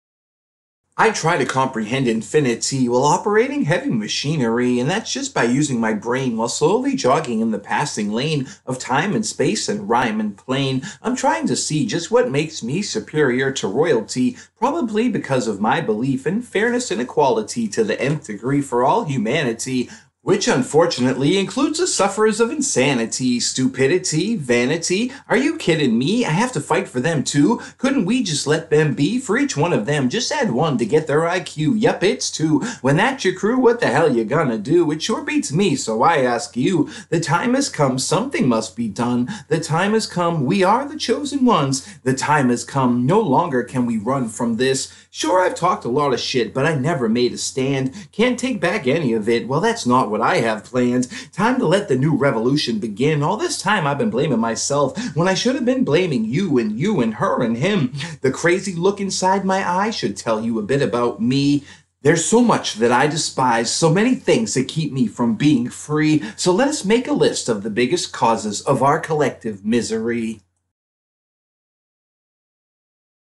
heavy machines - (turn this into a song!)
HEAVY MACHINES by Tom Jensen
I try to comprehend infinity
While operating heavy machinery
And that’s just by using my brain

While slowly jogging
in the passing lane
Of time and space
and rhyme and plane
I’m trying to see
just what makes me
Superior to royalty
probably because of my belief

In fairness and equality
to the nth degree for all humanity

Which unfortunately includes
the sufferers of insanity,
stupidity, vanity…

I have to fight for them, too?

Couldn’t we just let them be?

For each one of them
just add one
to get their I.Q.
Yup…. it’s two…

When that’s your crew
what the hell you gonna do?
It sure beats me, so I ask you…

Something must be done
The time has come
We are the chosen ones

No longer can we run from this

Sure I talked a lot of sh*t
But I never made a stand
Can’t take back any of it
Well that’s not what I have planned

Time to let the new
revolution begin…

I’ve been blaming myself

When I should have been
blaming you and you…
And her and him

This crazy look inside my eye
Should tell you a little bit about me

There’s so much that I despise
So many things that keep me
from being free

So let us make a list
of the biggest causes
Of our collective misery…